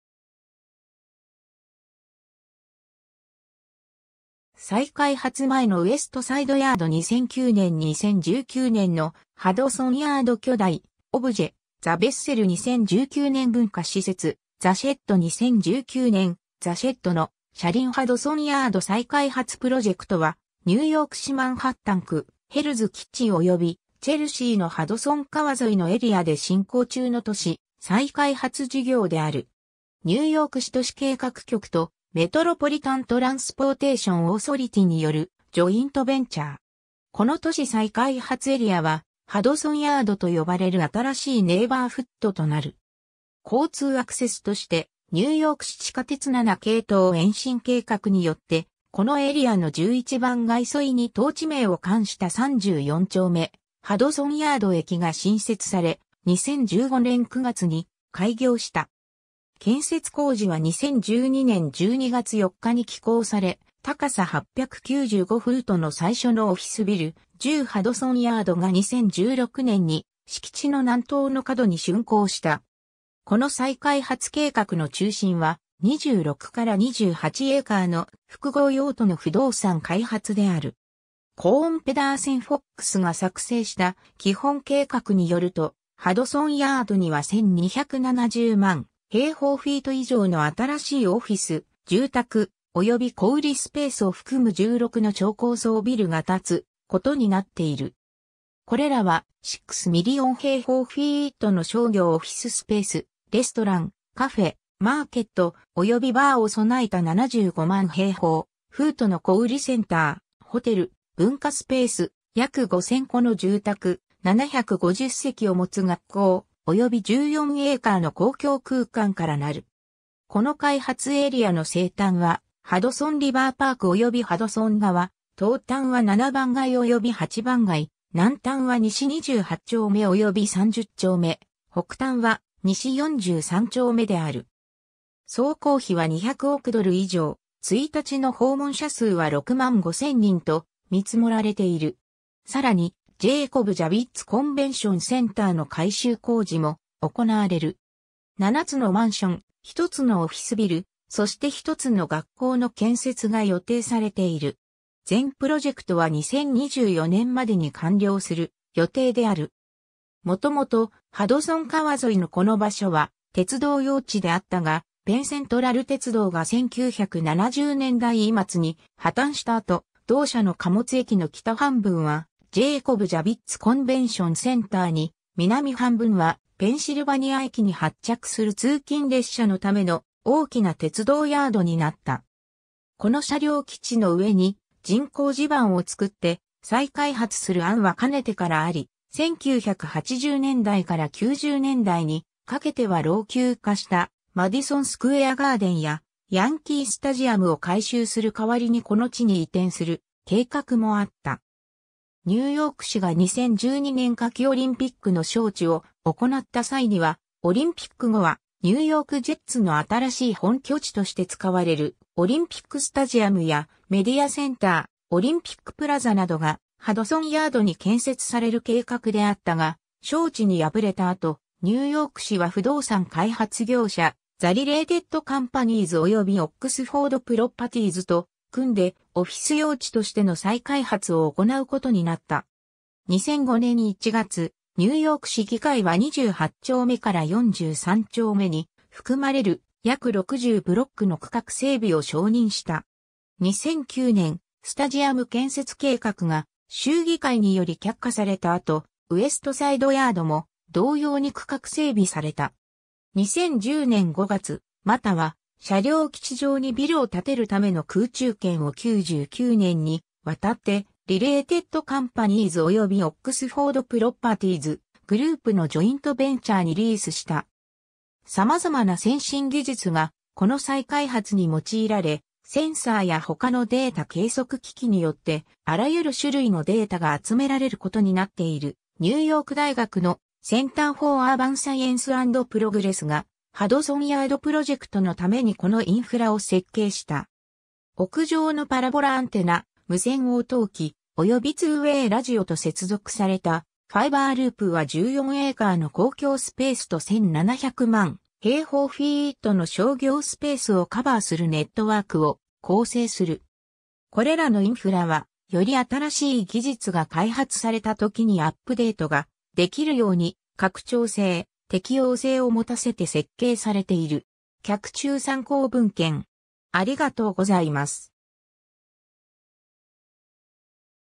ハドソン・ヤード
再開発前のウエスト・サイド・ヤード (en)　2009年 2019年のハドソン・ヤード 巨大オブジェ「The Vessel」 2019年 文化施設「The Shed」2019年 「The Shed」の車輪 ハドソン・ヤード再開発プロジェクト (Hudson Yards Redevelopment Project) はニューヨーク市マンハッタン区ヘルズ・キッチンおよびチェルシーのハドソン川沿いのエリアで進行中の都市再開発事業である。
ニューヨーク市都市計画局とメトロポリタン・トランスポーテーション・オーソリティ (MTA) によるジョイント・ベンチャー。
 この都市再開発エリアはハドソン・ヤード (Hudson Yards) と呼ばれる新しいネイバーフッドとなる。
交通アクセスとしてニューヨーク市地下鉄7系統延伸計画によって、このエリアの11番街沿いに当地名を冠した34丁目-ハドソン・ヤード駅が新設され、2015年9月に開業した。
 建設工事は2012年12月4日に起工され、高さ895-フート (273 m)の最初のオフィスビル「10 ハドソン・ヤード」が2016年に敷地の南東の角に竣工した。
 この再開発計画の中心は、26-28エーカー (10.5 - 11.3 ha)の複合用途の不動産開発である。
コーン・ペダーセン・フォックスが作成した基本計画（マスタープラン）によると、ハドソン・ヤードには12,700,000平方フィート (1,180,000 m2)以上の新しいオフィス、住宅、および小売スペースを含む16の超高層ビルが建つことになっている。
これらは、six million平方フィート (560,000 m2)の商業オフィススペース、レストラン、カフェ、マーケット、およびバーを備えた750,000-平方フート (70,000 m2)の小売センター、ホテル、文化スペース、約5,000戸の住宅、750席を持つ学校、および14エーカー (5.7 ha)の公共空間からなる。
 この開発エリアの西端はハドソン・リバー・パークおよびハドソン川、東端は7番街および8番街、南端は西28丁目および30丁目、北端は西43丁目である。
総工費は200億ドル以上、1日の訪問者数は65,000人と見積もられている。
 さらに、ジェイコブ・ジャビッツ・コンベンション・センターの改修工事も行われる。
 7つのマンション、1つのオフィスビル、そして1つの学校の建設が予定されている。
 全プロジェクトは2024年までに完了する予定である。
 もともとハドソン川沿いのこの場所は鉄道用地であったが、ペン・セントラル鉄道が1970年代末に破綻した後、同社の貨物駅の北半分はジェイコブ・ジャビッツ・コンベンション・センターに、南半分はペンシルバニア駅に発着する通勤列車のための大きな鉄道ヤード（MTAのJohn D. Caemmererウエスト・サイド車両基地）になった。
この車両基地の上に人工地盤を作って再開発する案はかねてからあり、1980年代から90年代にかけては老朽化したマディソン・スクエア・ガーデンやヤンキースタジアムを改修する代わりにこの地に移転する計画もあった。
 ニューヨーク市が2012年夏季オリンピックの招致を行った際には、オリンピック後はニューヨーク・ジェッツの新しい本拠地として使われるオリンピックスタジアム（ウエスト・サイド・スタジアム）やメディアセンター、オリンピックプラザなどがハドソン・ヤードに建設される計画であったが、招致に敗れた後、ニューヨーク市は不動産開発業者The Related CompaniesおよびOxford Propertiesと組んでオフィス用地としての再開発を行うことになった。
 2005年1月、ニューヨーク市議会は28丁目から43丁目に含まれる約60ブロックの区画整備を承認した。
 2009年、スタジアム建設計画が州議会により却下された後、ウエスト・サイド・ヤードも同様に区画整備された。
 2010年5月、MTAは車両基地上にビルを建てるための空中権を99年に渡ってRelated CompaniesおよびOxford Properties Groupのジョイントベンチャーにリースした。
 様々な先進技術がこの再開発に用いられ、センサーや他のデータ計測機器によって、あらゆる種類のデータが集められることになっている： ニューヨーク大学のCenter f...